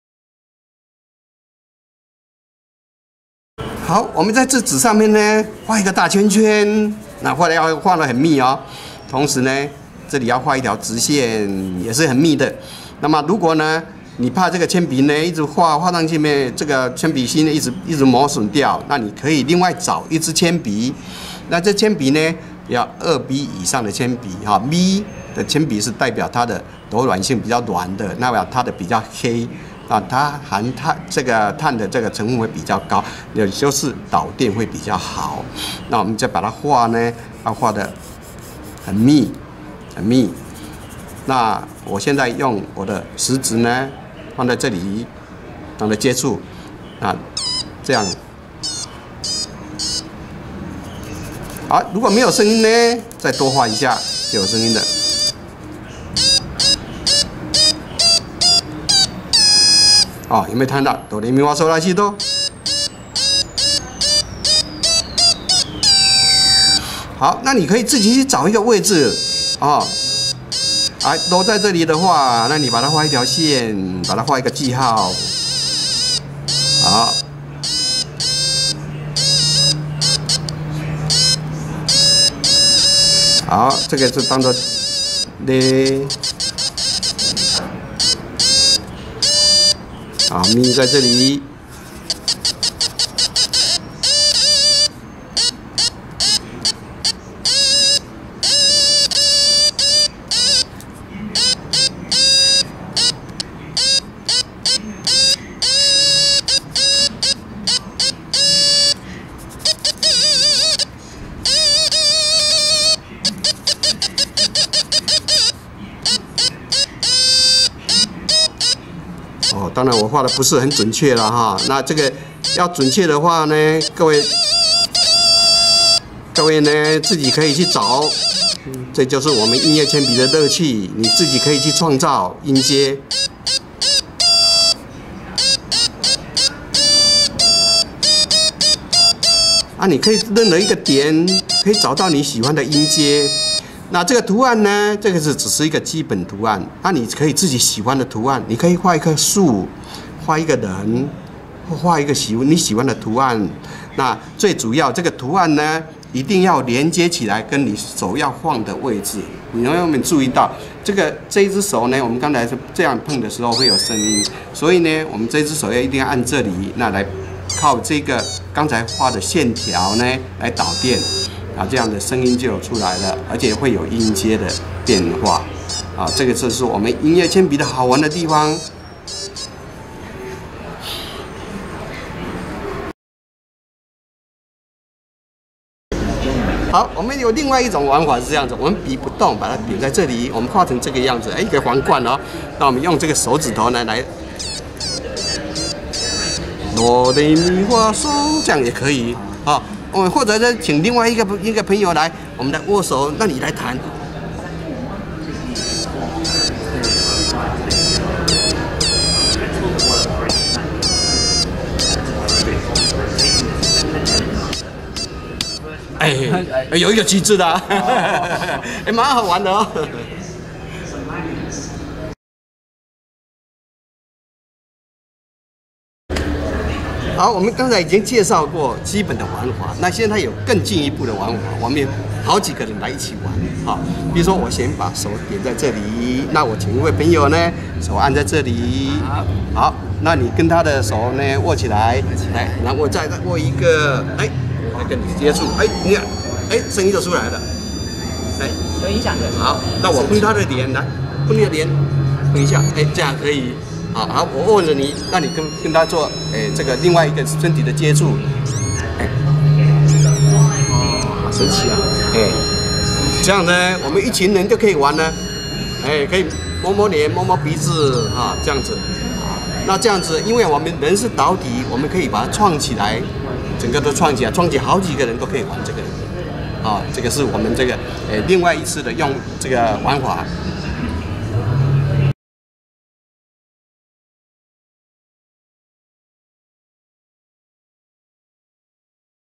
電子音樂鉛筆有趣的玩法
電子音樂鉛筆不只是會發出聲音，透過畫在紙上的碳粉線條找出音階的變化後，還可以彈奏出一首音樂!
更有趣的是將許多人手牽手連結起來，透過導體的連結形成的迴路後，一樣會讓鉛筆發出聲音，甚至還可以觸碰臉部或手部皮膚來玩創聲音。還有其他玩法嗎?相信一定有很多趣味的體驗方式等著您來創作喔!